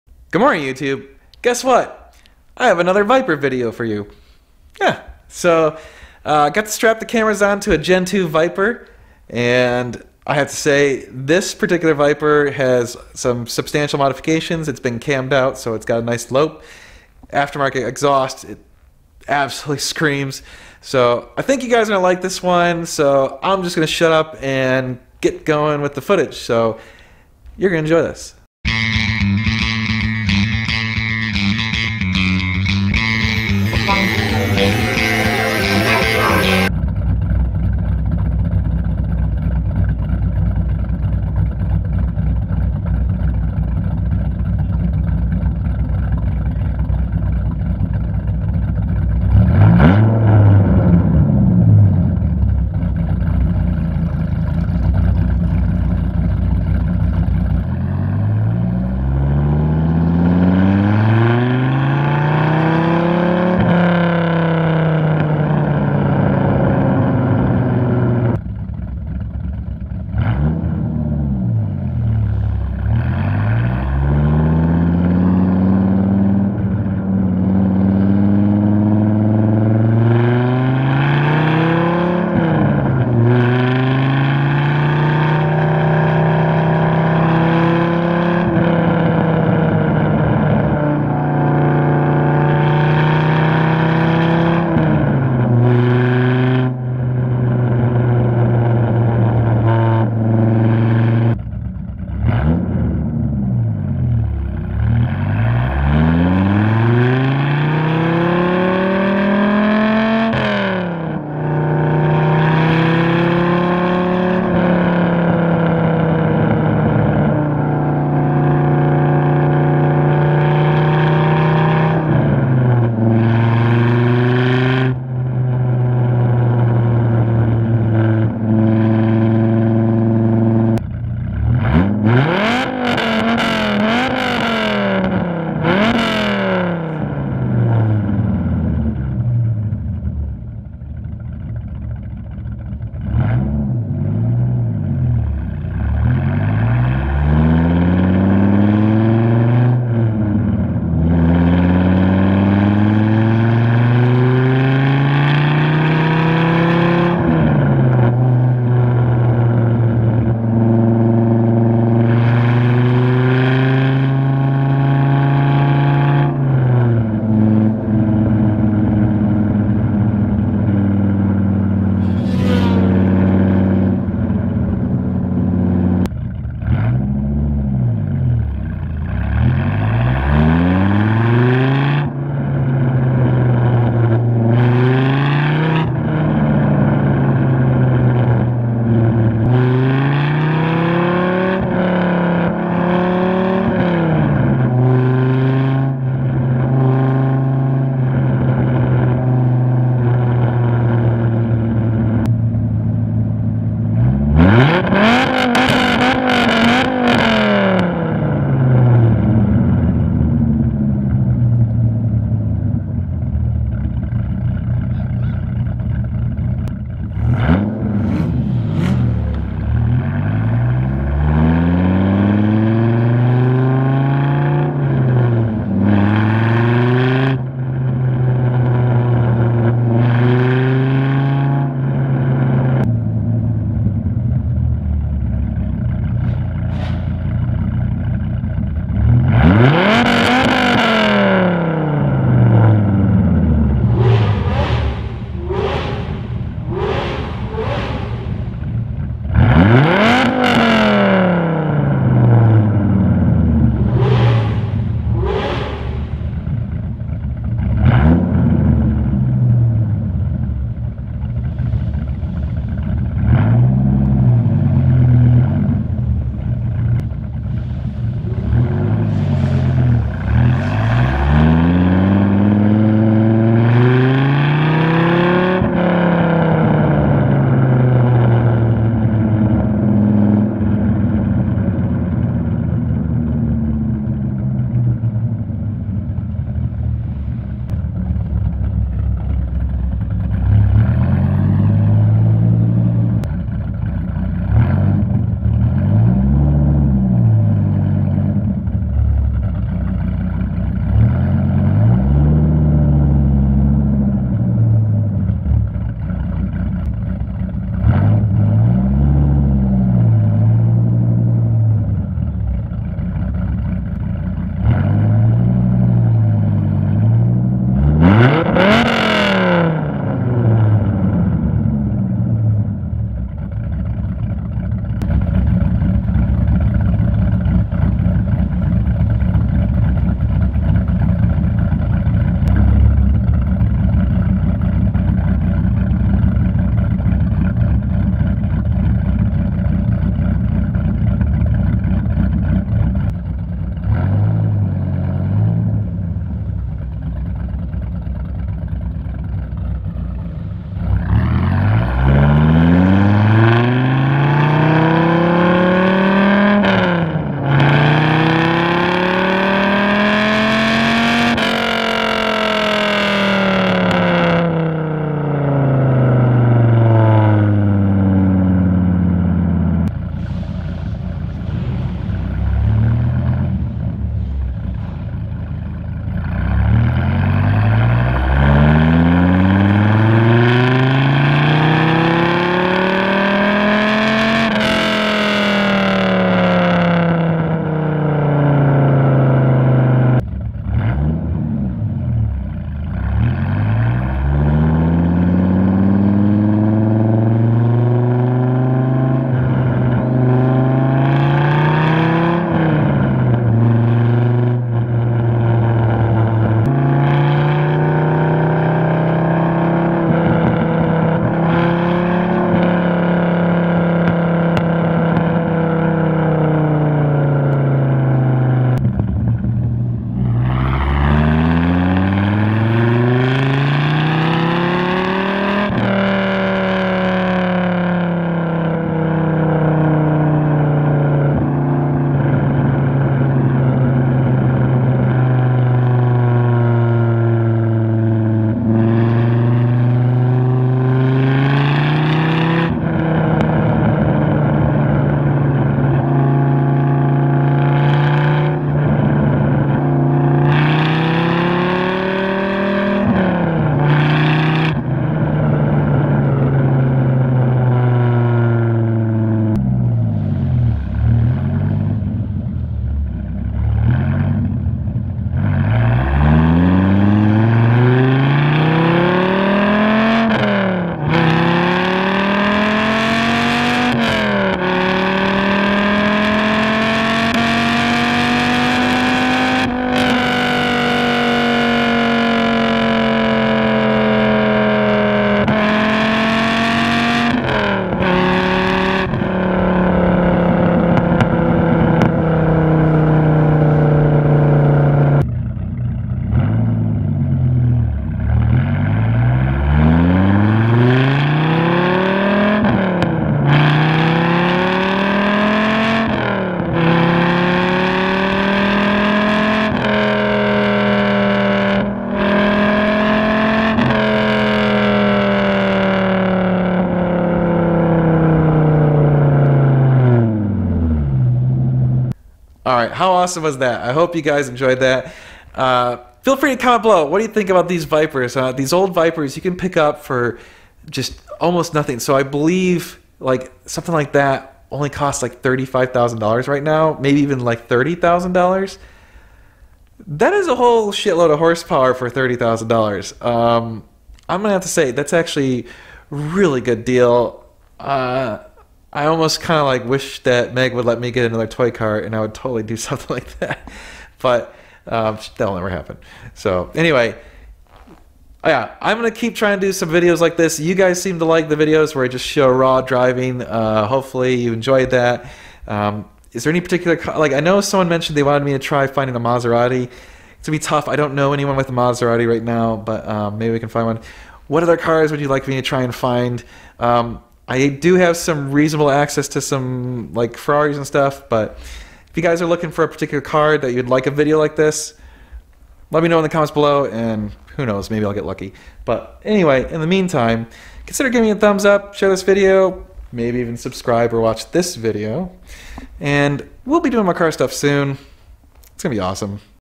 Gen 2 Dodge Viper GTS raw sound ride along
Raw sound of a Gen 2 Dodge Viper  GTS out for a drive.

Some of the Hardware I use to make these videos...
POV Video Glasses
GoPro Hero 5
GoPro Suction Mount
GoPro Mic Adapter
Rode VideoMic Pro
Rode VideoMic